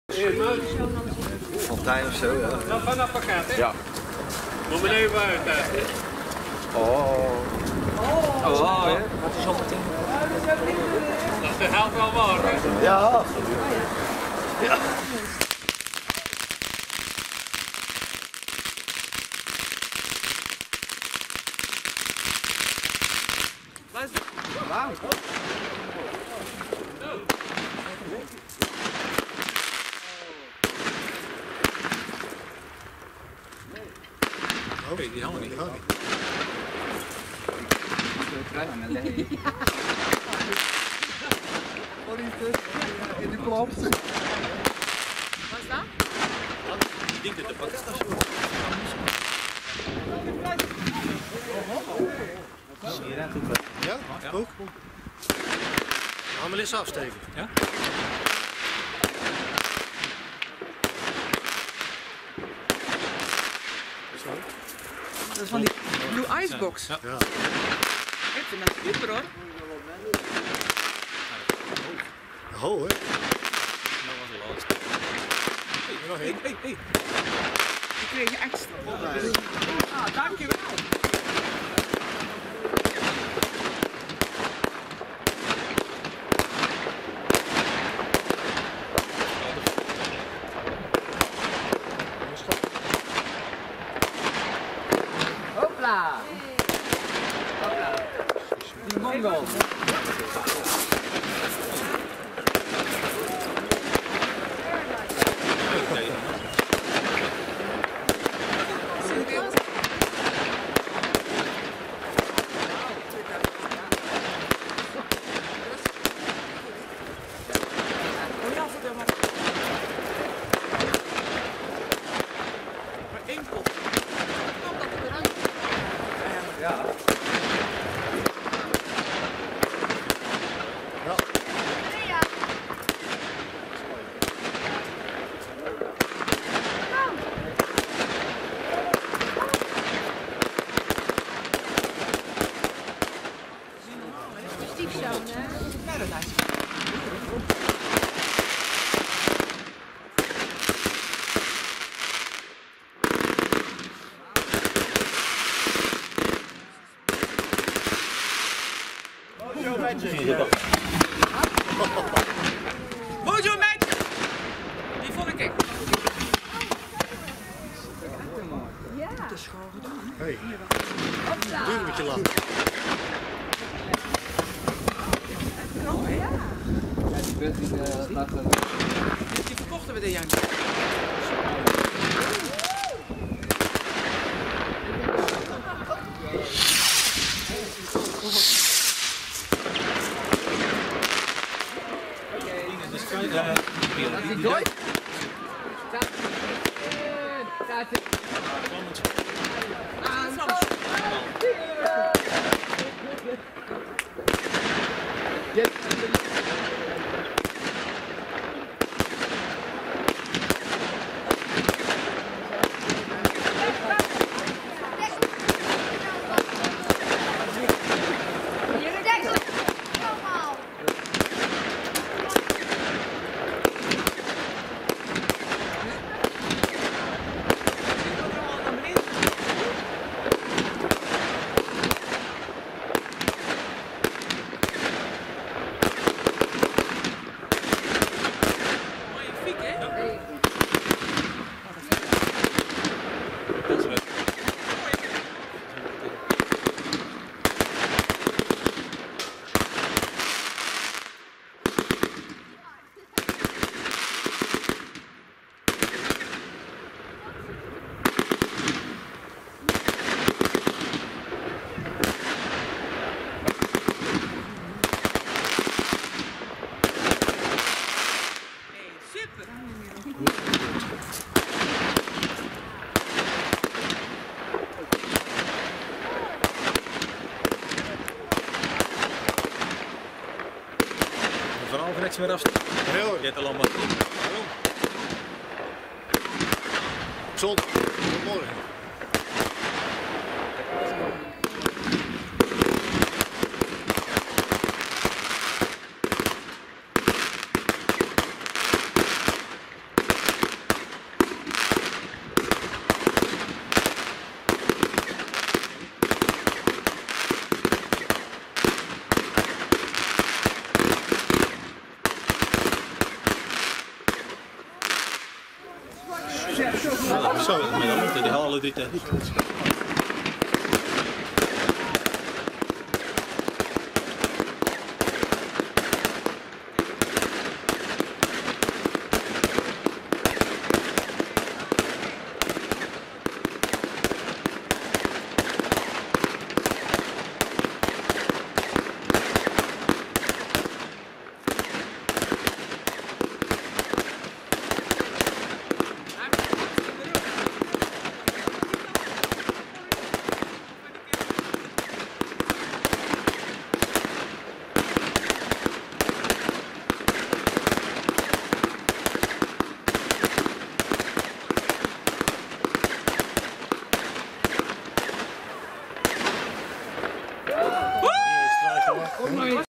18:00 show bij Partymax Vuurwerk 31-12-2015
Vuurwerk showtje om 6 uur bij Partymax Vuurwerk op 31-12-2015 te Vogelwaarde